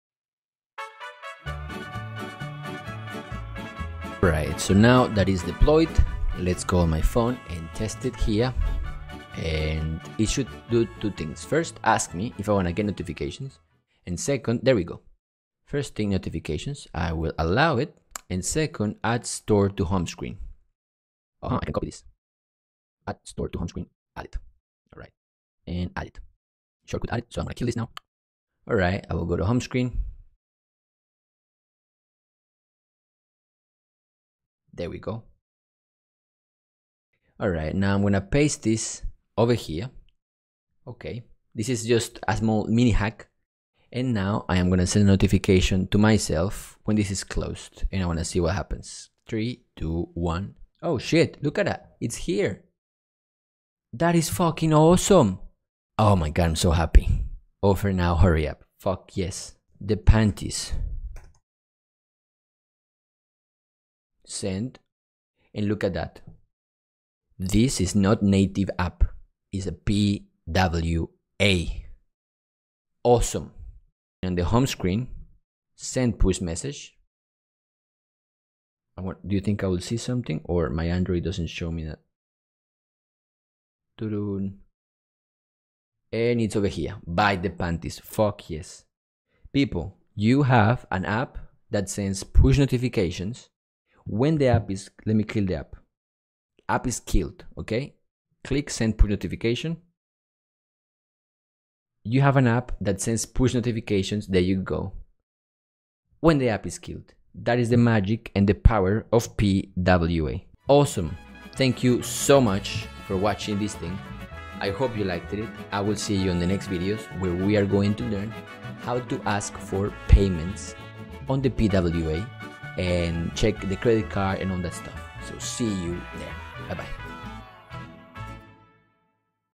PWA 웹앱 푸시 알림 테스트하기!? 이거슨 PWA의 뽜와! (Testing Push Notification of PWA)
노마드 스토어를 만들었어요! PWA로!!
구현한 푸시 알림을 테스트 해봤습니다.
Camera: Galaxy S9+
Location: Seoul, S.Korea
Learn to Code with Nicolas!